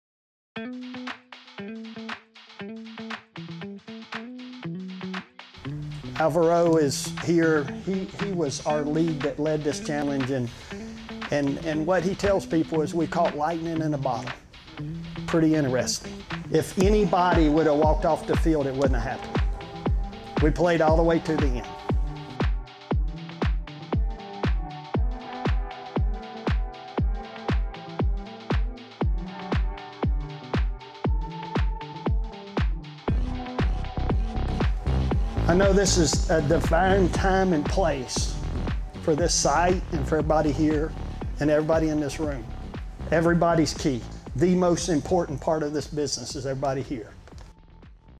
Vertex Day 2022
Check out our short recap of Vertex Day, where we welcomed and celebrated the Mobile Site to Vertex Energy!